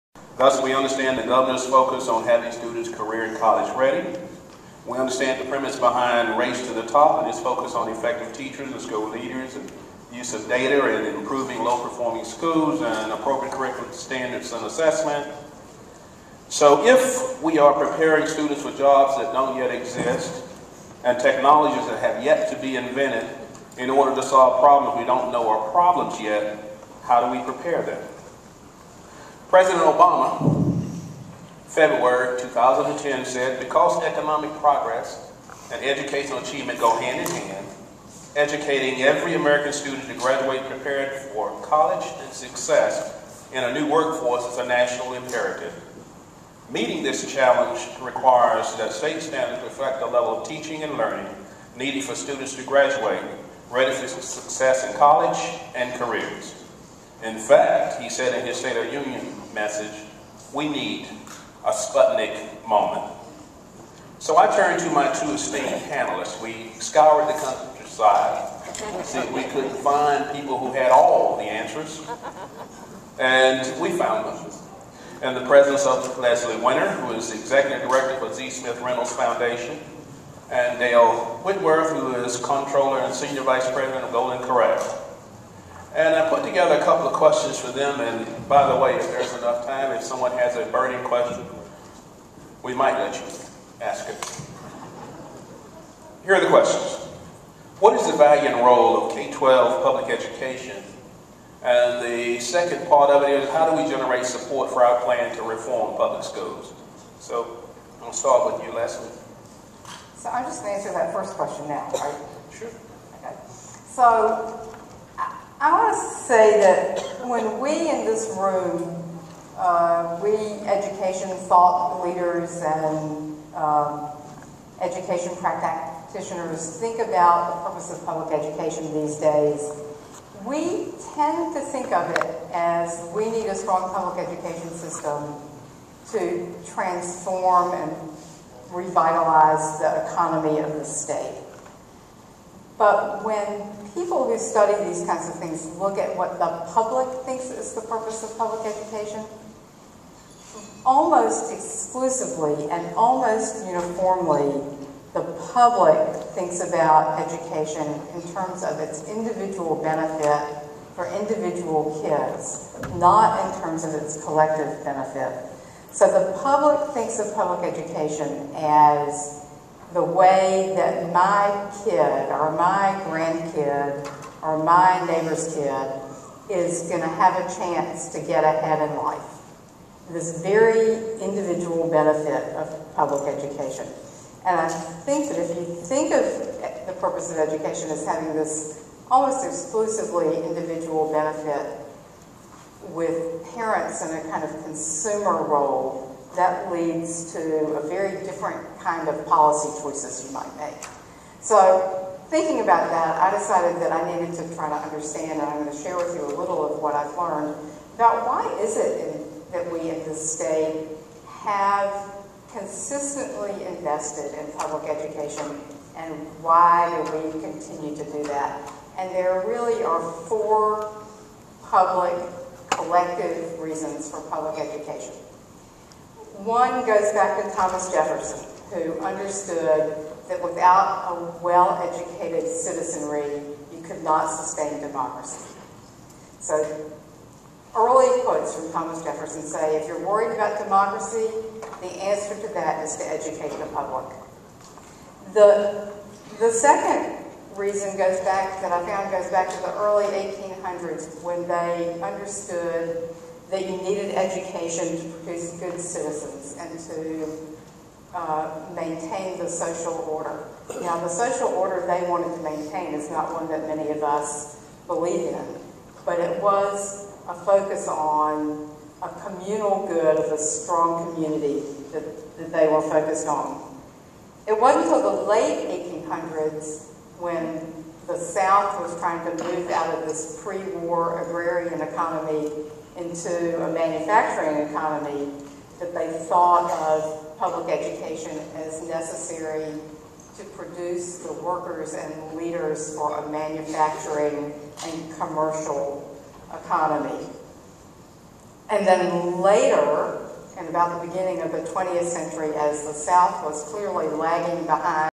Panel Discussion: Candid Conversations about Public Education in NC (Part 1)
Bill McNeal, executive director of the North Carolina Association of School Adminstrators, moderates a discussion with Leslie Winner, executive director of the Z. Smith Reynolds Foundation, and Dale Whitworth, senior vice president of Golden Corral Corporation, at the "Many Voices, One Goal" conference. (Part 1 of 4)